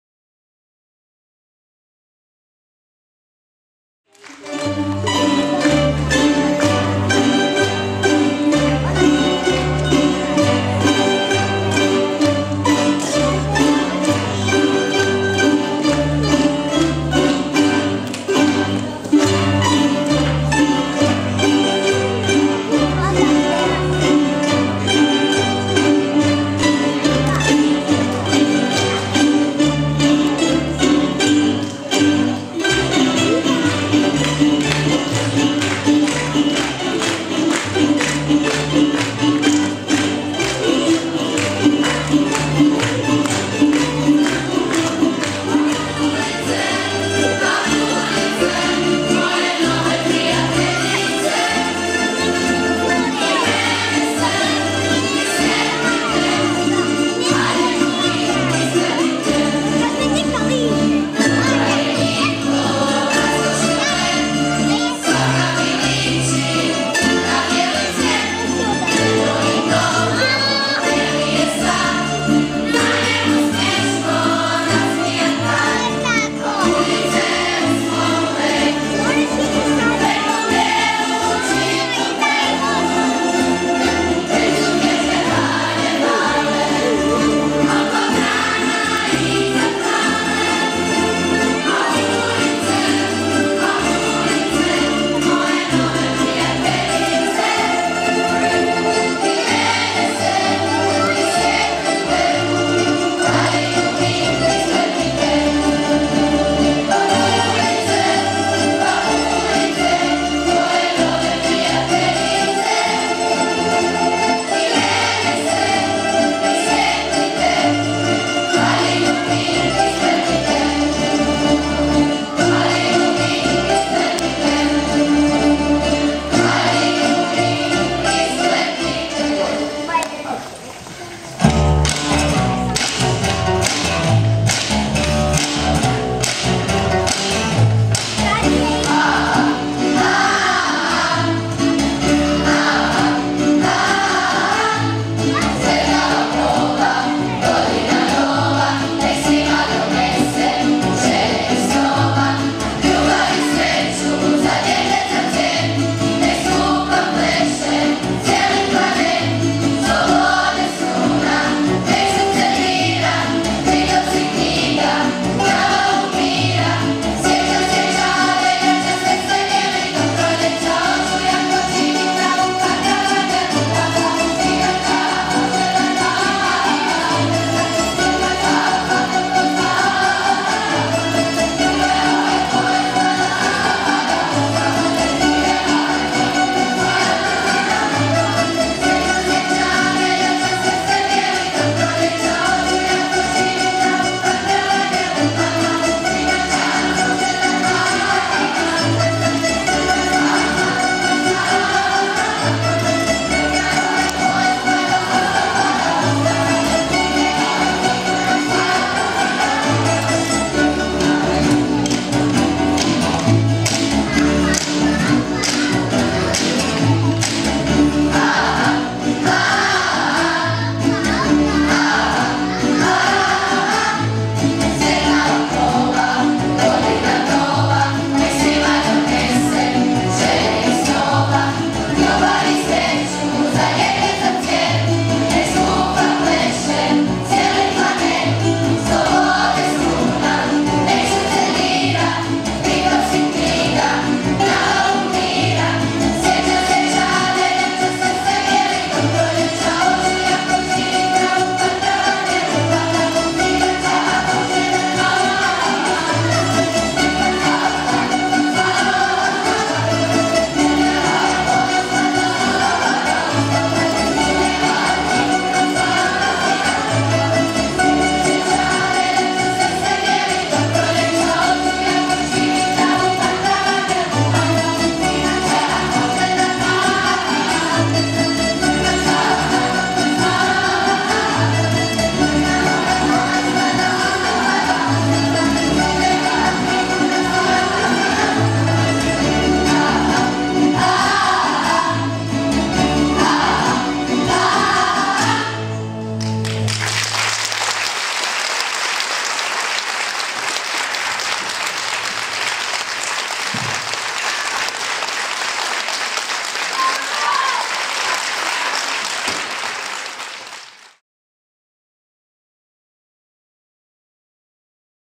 Božićni koncert
Božićni koncert učenika OŠ Popovača održan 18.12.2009.